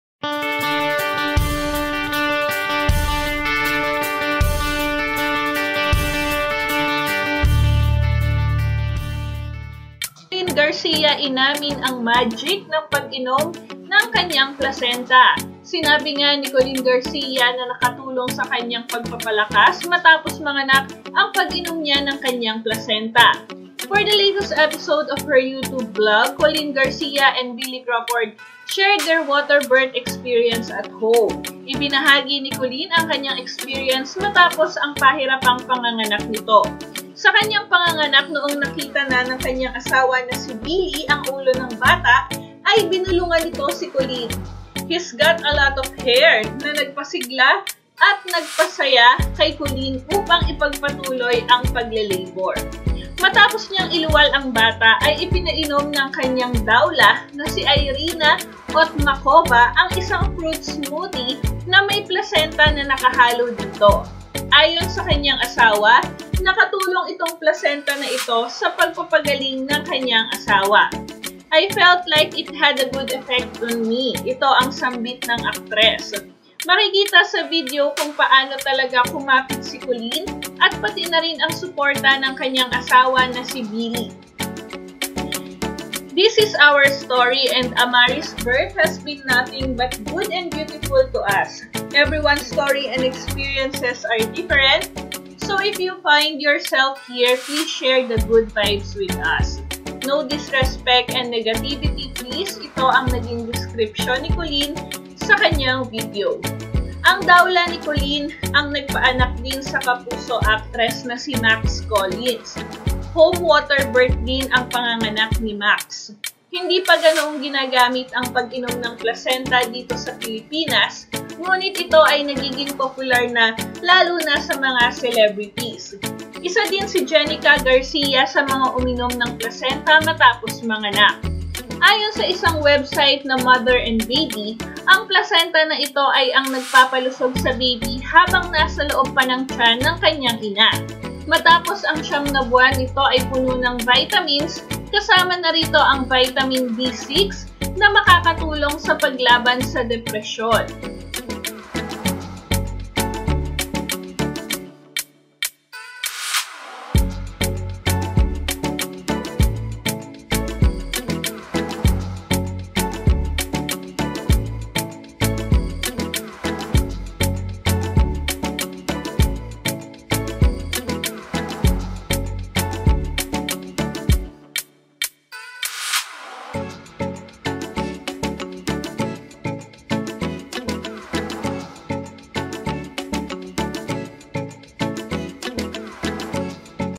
COLEEN GARCIA, INAMIN ang MAGIC ng PAG-INOM nya ng kanyang PLACENTA matapos MANGANAK
Ganito pala ang nagagawa ng placenta sa isang bagong panganak na ina.